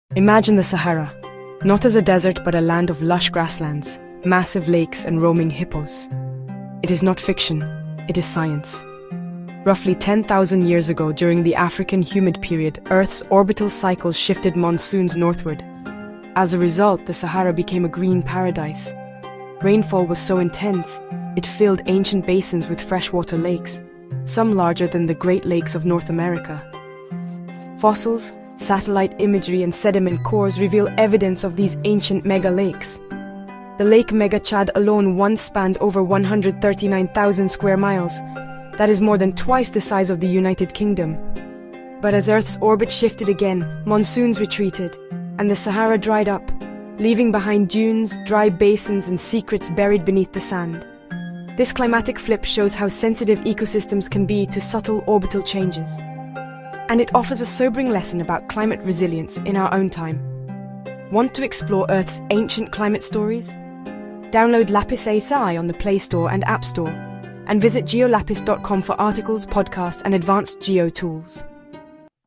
Rainfall Once Filled the Sahara with Lakes Bigger Than the Great Lakes
The Sahara was once green, with lakes bigger than the Great Lakes. What changed — and what can it teach us about climate?